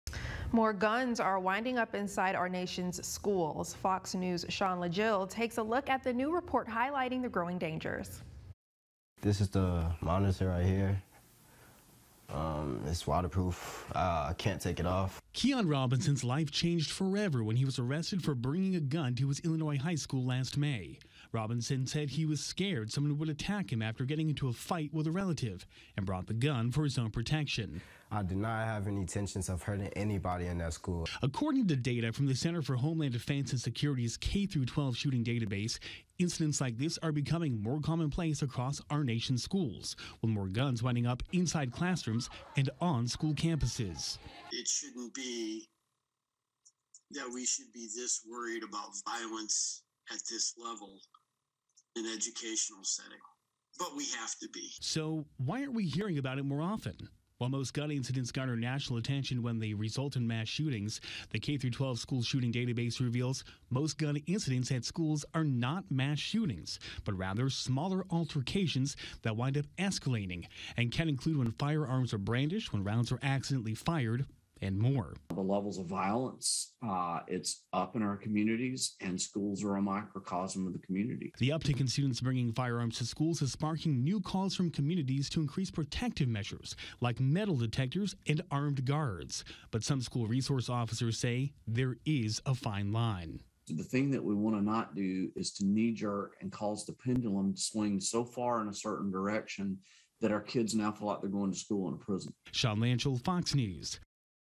Data shows that guns are increasing within schools.
The Center for Homeland Defense and Security K-12 shooting database shows that incidents with guns in school are becoming more common.